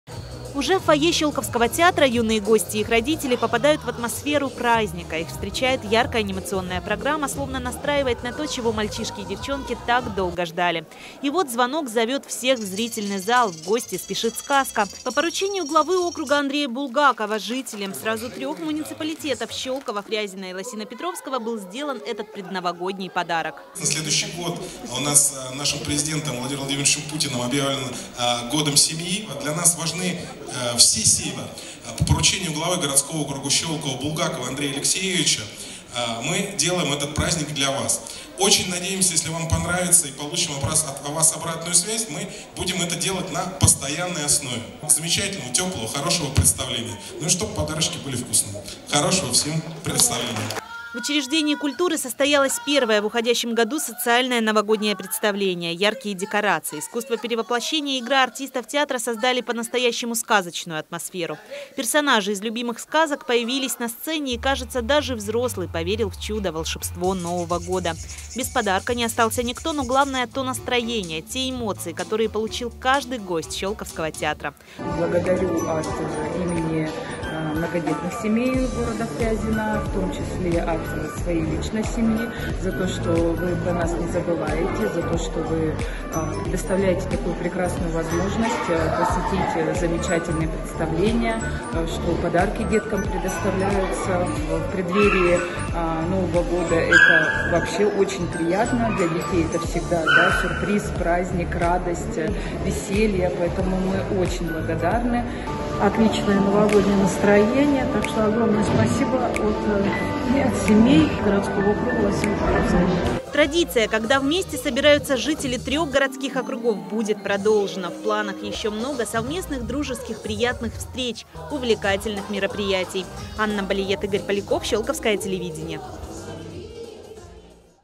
Ёлка для трёх городских округов
В Театрально-концертном центре «Щёлковский театр» закружился первый хоровод, зажглись огни на новогодней ёлке – представление для юных жителей сразу трёх городских округов дали артисты. Первая социальная ёлка состоялась в стенах учреждения культуры